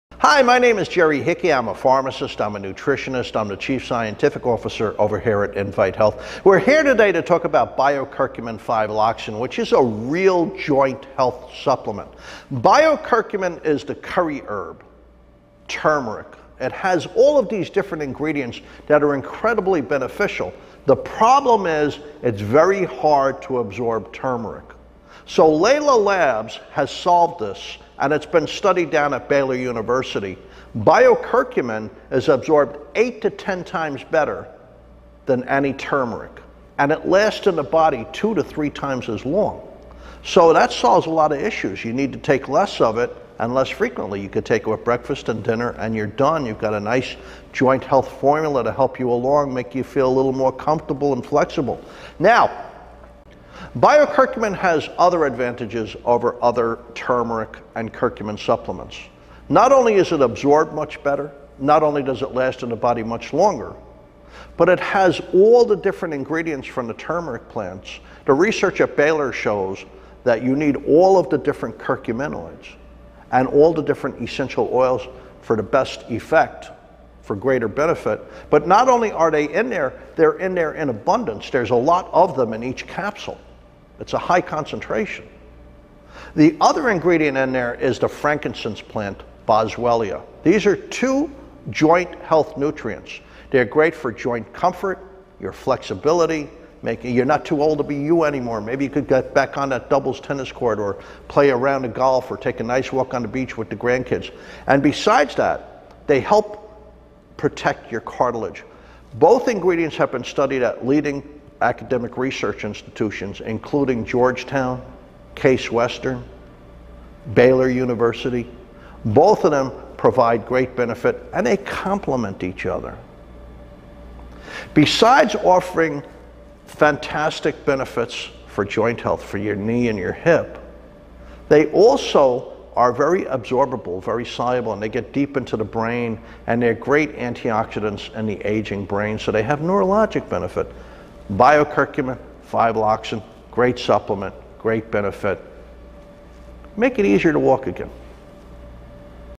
Jerry Hickey: Bio-Curcumin® & 5-Loxin™ Supplement
Jerry Hickey, R. Ph. discusses InVite Health's Bio-Curcumin® & 5-Loxin™ supplement, which supports the body’s everyday inflammatory response and hinders normal, aging-related aches and pains.